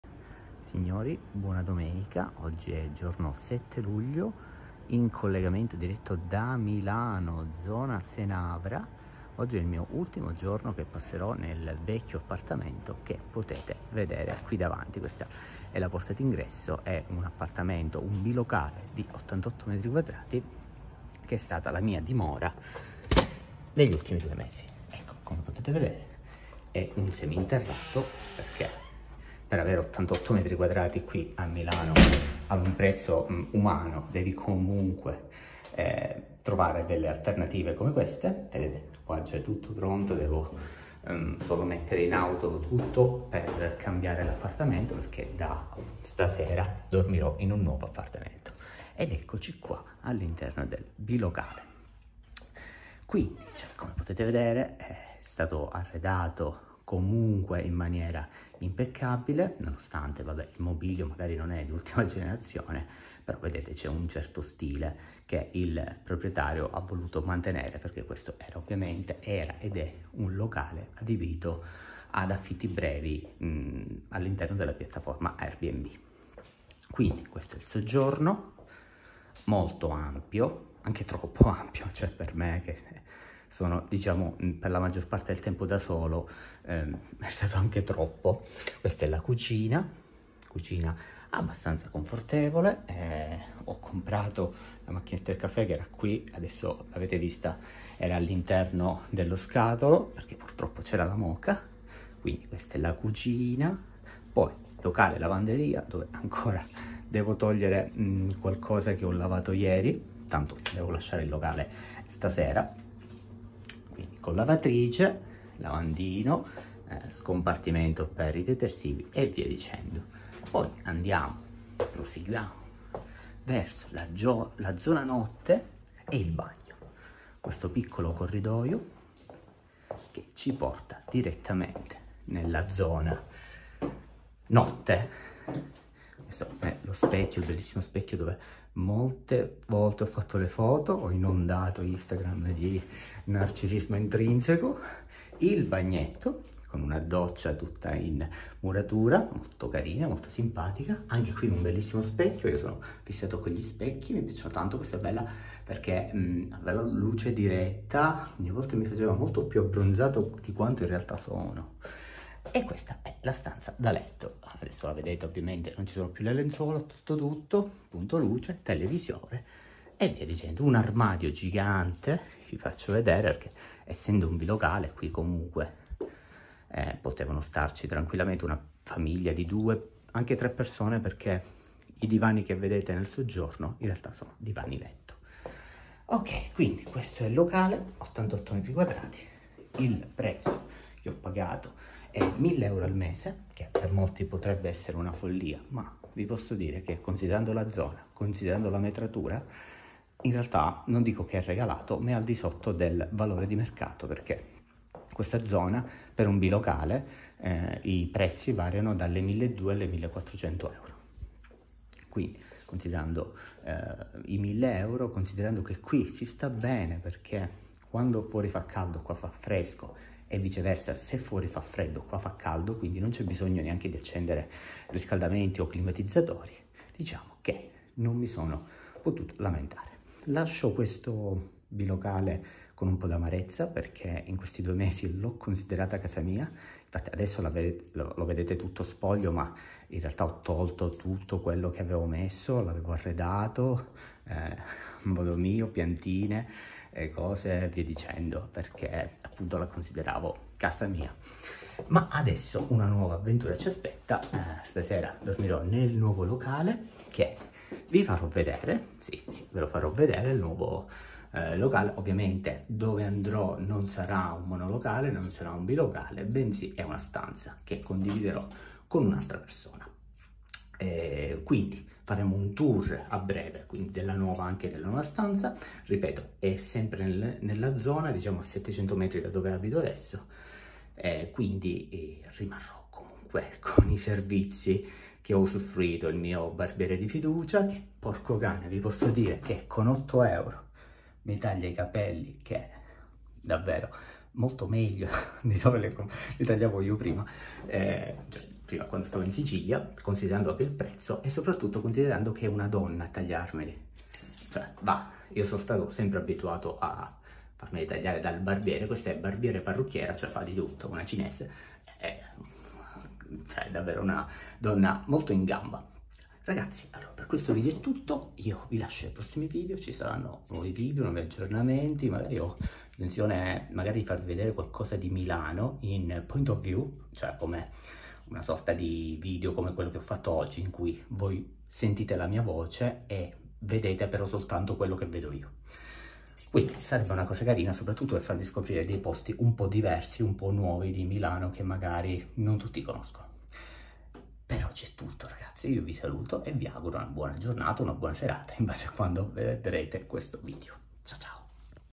AFFITTI a MILANO | il mio vecchio Bilocale da 88mq2
Tour del mio vecchio appartamento di Milano che a malincuore lascio per trasferirmi altrove